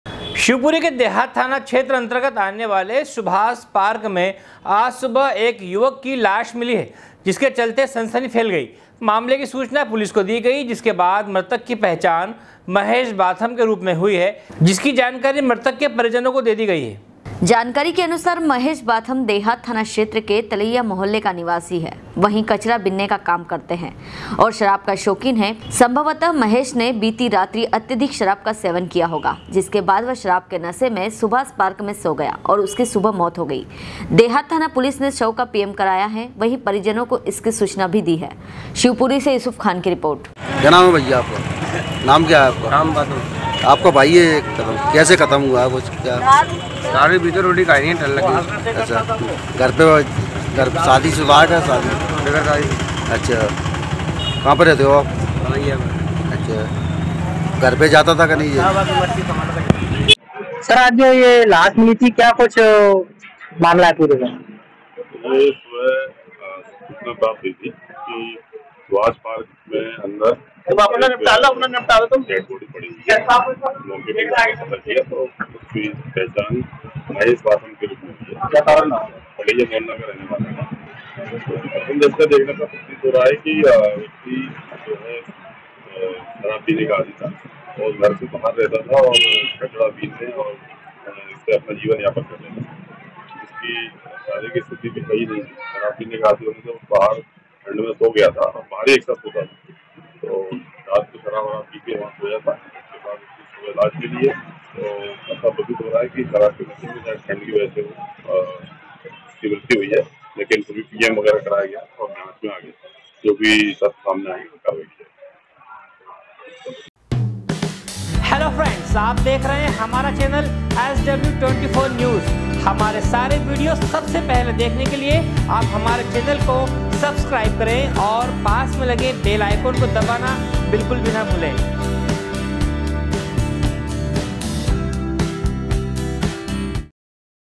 शिवपुरी के देहात थाना क्षेत्र अंतर्गत आने वाले सुभाष पार्क में आज सुबह एक युवक की लाश मिली है
शिवपुरी के देहात थाना क्षेत्र अंतर्गत आने वाले सुभाष पार्क में आज सुबह एक युवक की लाश मिली है जिससे सनसनी फैल गई मामले की सूचना पुलिस को दी गई जिसके बादे मृतक की पहचान महेश बाथन के रूप मे हुई है जिसकी जानकारी मृतक के परिजनों को दी गई.
शिवपुरी युसूफ खान की रिपोर्ट